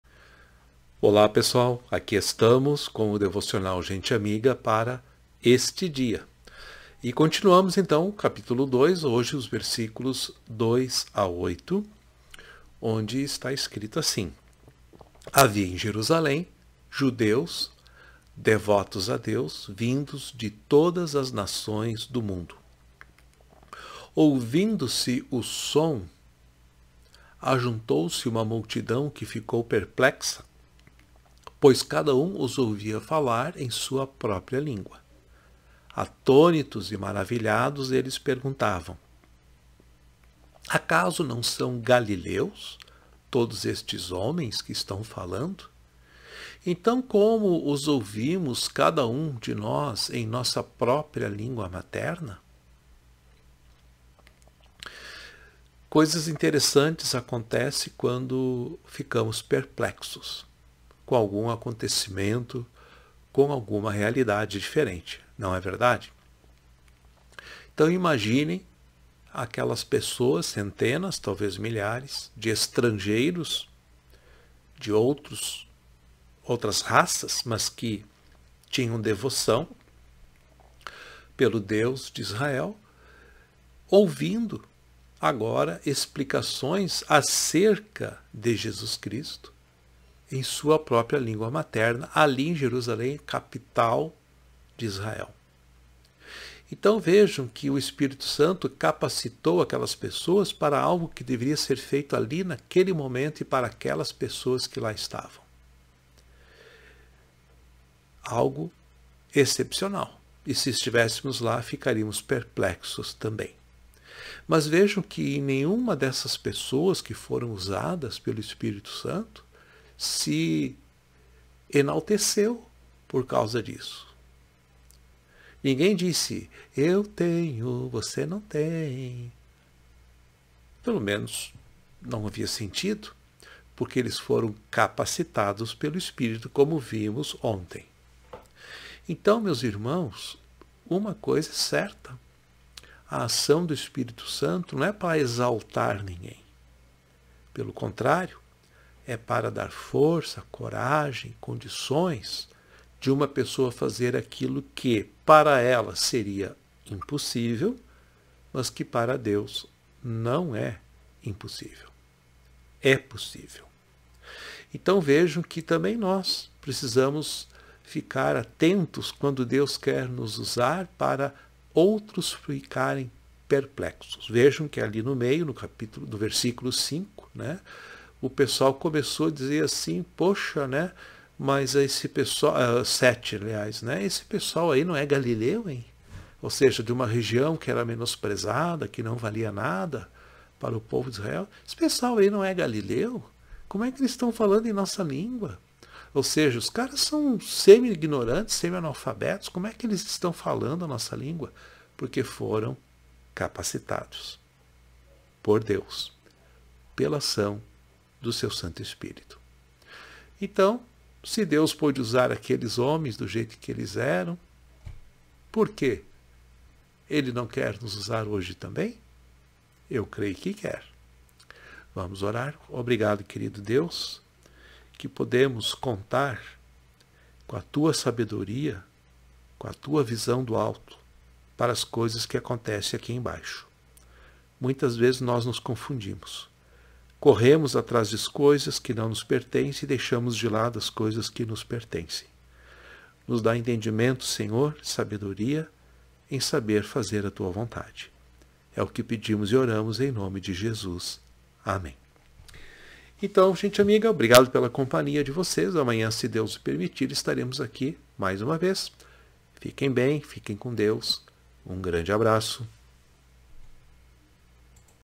Devocional GENTE AMIGA Atos Nº 10 - PERPLEXOS! Atos 02.05-08 4ª feira, 27/09/2023
Devocional GENTE AMIGA
Atos Nº 10 - PERPLEXOS!
Atos 02.05-08
4ª feira, 27/09/2023
Uma jornada pelo Livro de ATOS dos apóstolos
Pr. Jairo Gustavo Ferreira Cruz
Pastor emérito da IECLB
Igreja Evangélica de Confissão Luterana no Brasil